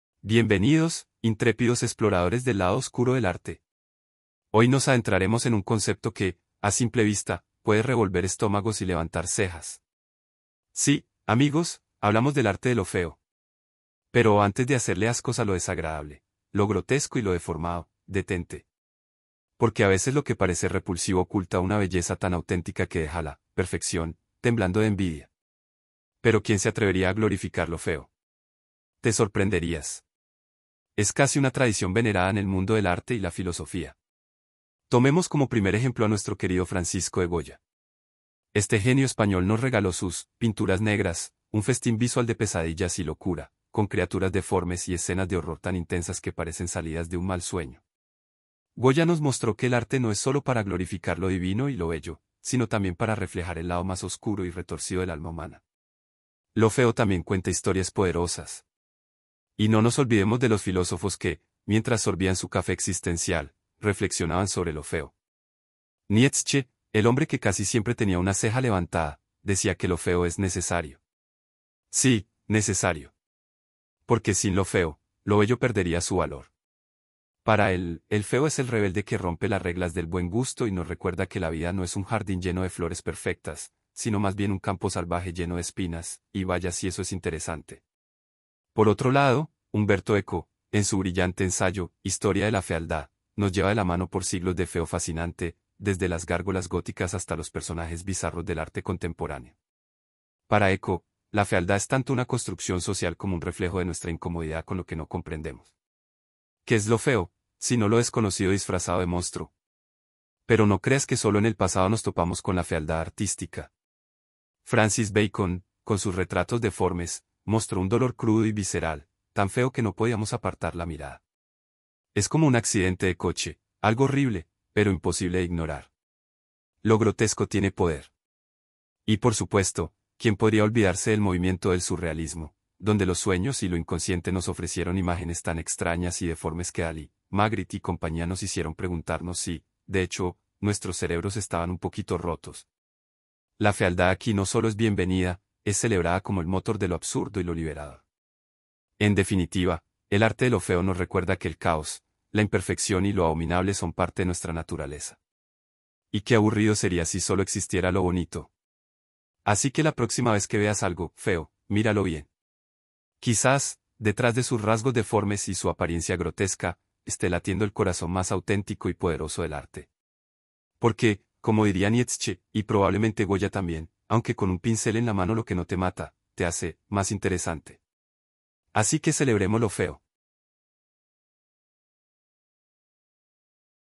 arte de lo feo
En definitiva, el arte de lo feo nos recuerda que el caos, la imperfección y lo abominable son parte de nuestra naturaleza. ¡Y qué aburrido sería si solo existiera lo bonito! Así que la próxima vez que veas algo "feo", míralo bien. Quizás, detrás de sus rasgos deformes y su apariencia grotesca, esté latiendo el corazón más auténtico y poderoso del arte.